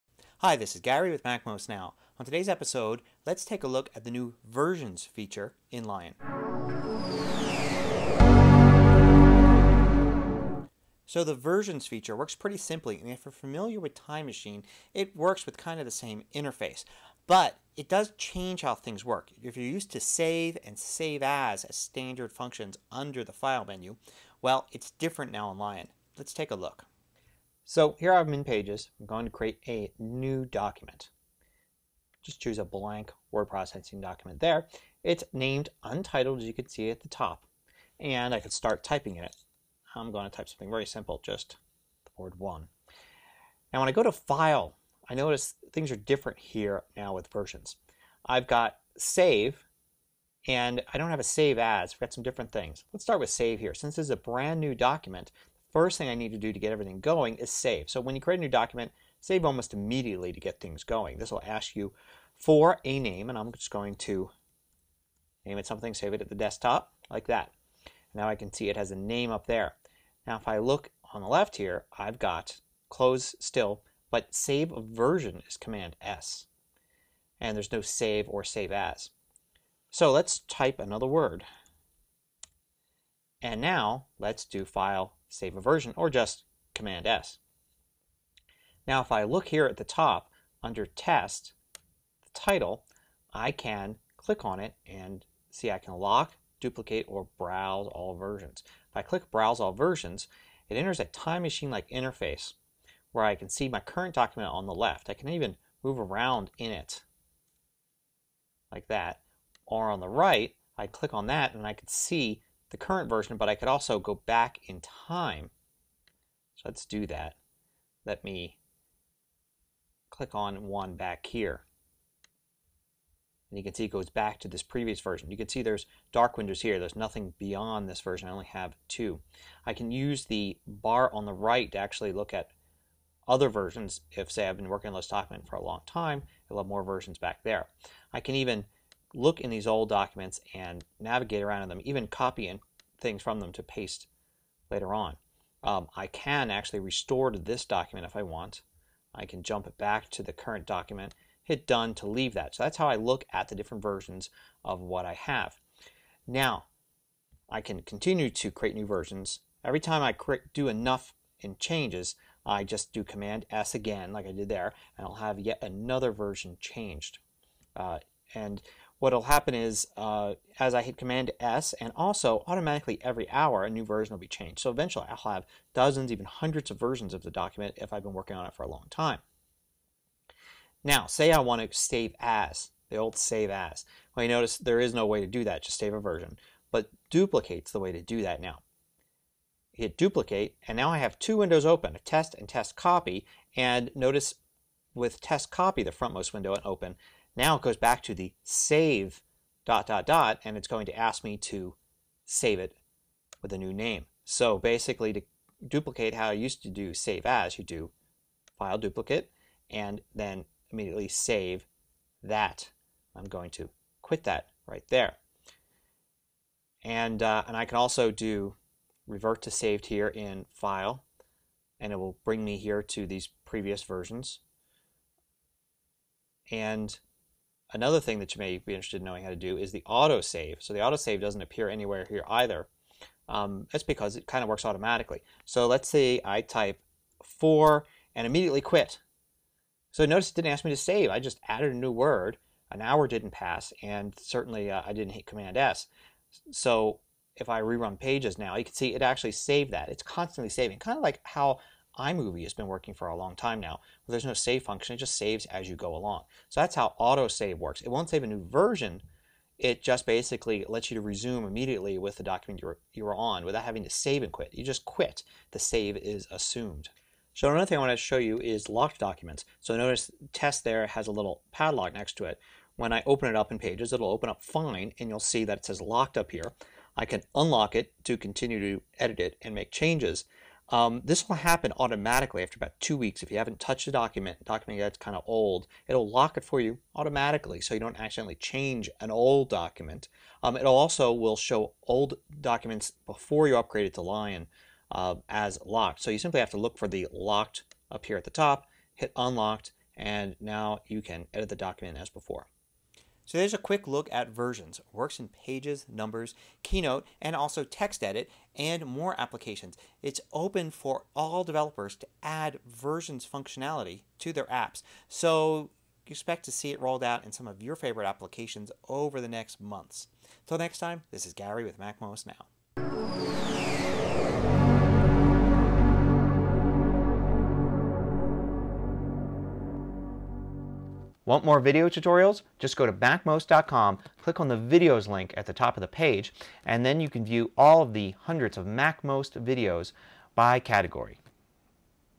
Lion Versions Basics (MacMost Now 585)
Learn the basics of Versions, a new feature in Lion that saves multiple copies of your documents as you work. Gone are the Save and Save As commands we have been used to for years. Learn how to use the new Save, Save a Version and Duplicate commands that give you a safety net while you work.